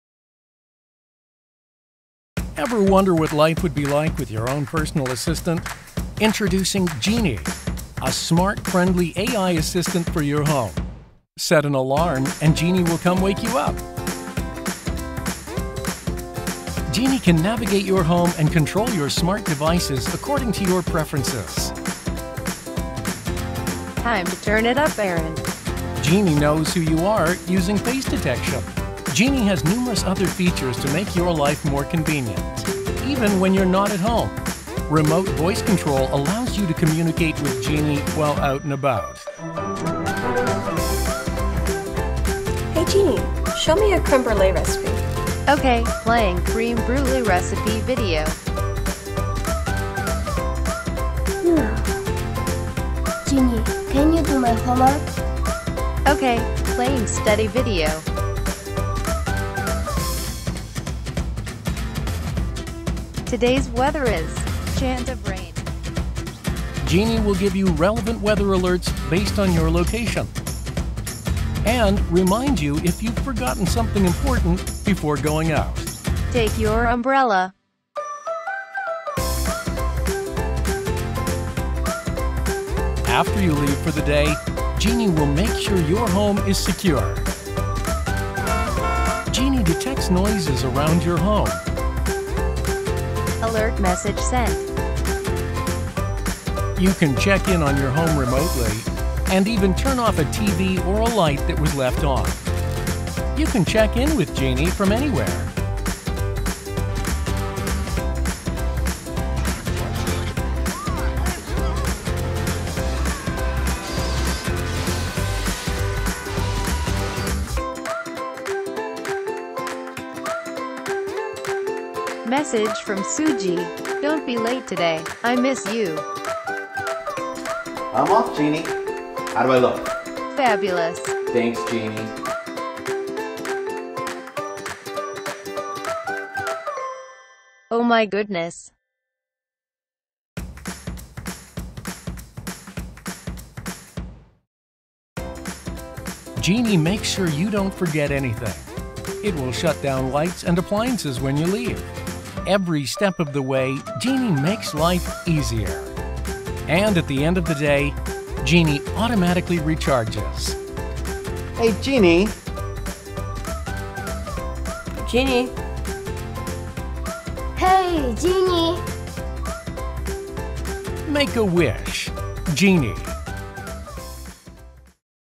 JINI - 당신의 집을 더 스마트하게, 인공지능 로봇 비서 JINI
JINI
시키는 일만 하는 로봇은 이제 안녕
스스로 할일을 찾아내고, 충전까지 해내는
정말 똑똑한 인공지능 로봇 비서 JINI